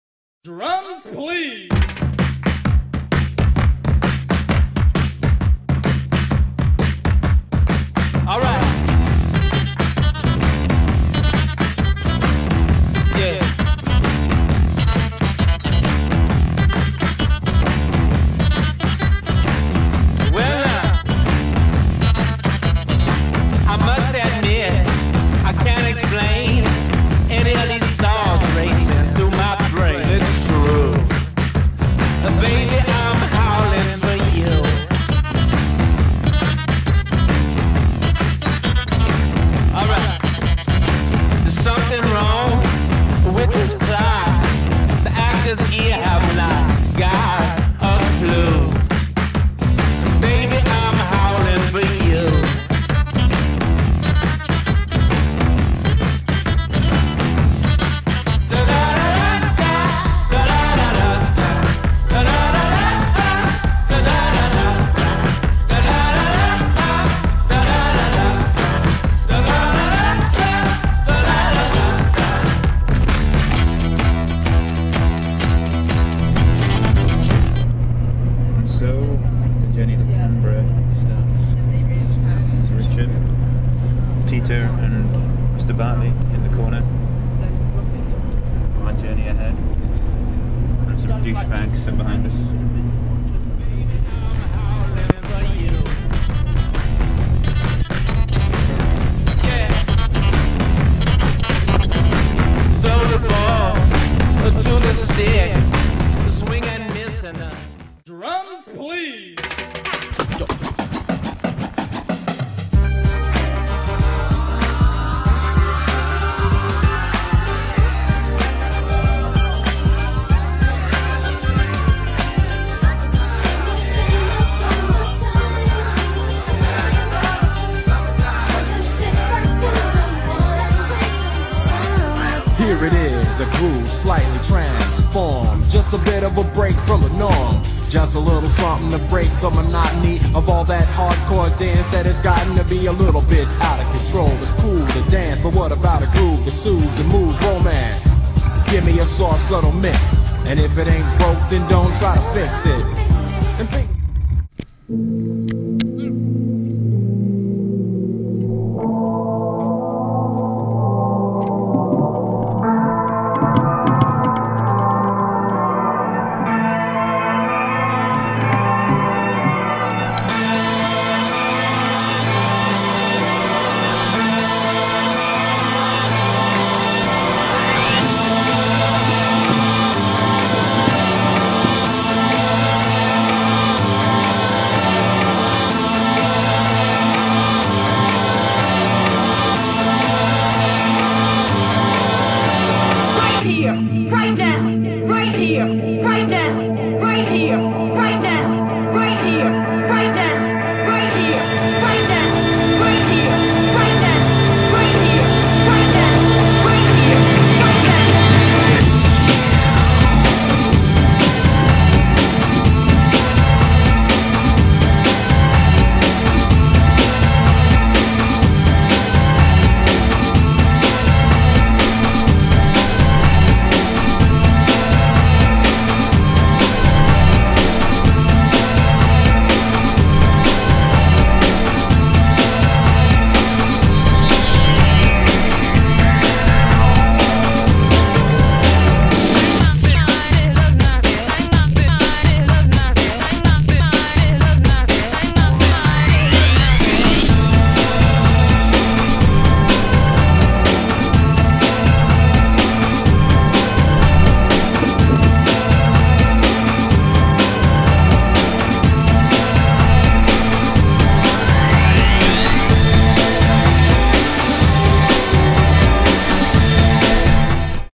WCP 1
A few snaps from World Cup I in Australia with a wee bit of video of the 1x...